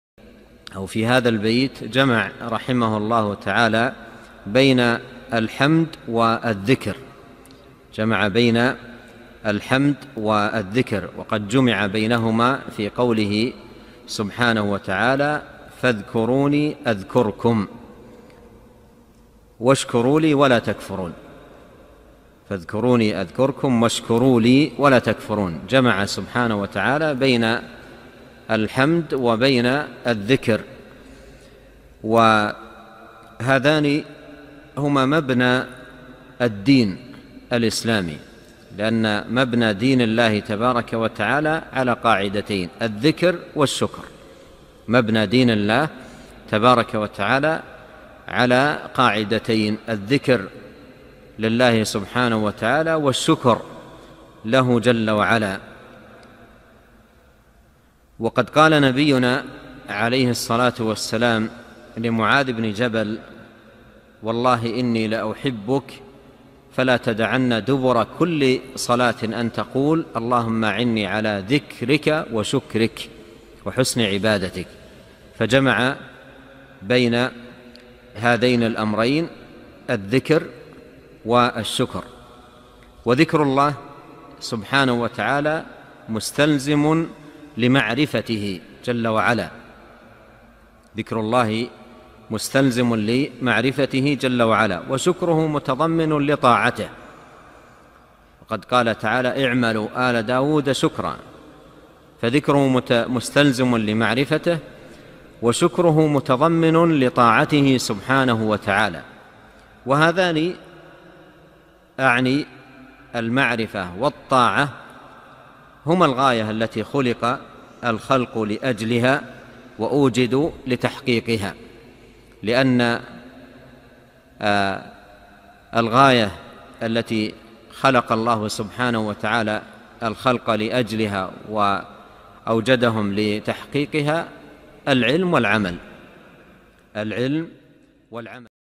Remembering Allah and Giving Thanks by Shaykh Abdur Razzaq Al Badr
Remembrance and Gratefulness - 2 important principles.

...And in this line of poetry (may Allah) have mercy upon him)
he gathered between praise and remembrance

And indeed it is also gathered in Allah ta'ala's speech:
"Therefore remember me (by praying, glorifying etc), I will
remember you, and be grateful to me, (for my countless favours
on you) and never be ungrateful. (2:152)

And these two (i.e. Dhikr & Shukr) are built upon the religion of Islam, because the religion of Allah is built upon two principles,
1. Dhikr and 2. Shukr

Allah's religion (jalla wa ala) is built upon two principles
Dhikr for Allah azza wa jal alone and shukr (giving thanks to him).

Indeed our prophet may peace and blessing be upon him said
to muadh ibn jabal may Allah be pleased with him:
"By Allah, Indeed I love for you that you don't leave off the following
at the end of every prayer, and you say
اللهم أعني على ذكرك وشكرك و حسن عبادتك

So gathered between these two affairs is dhikr and shukr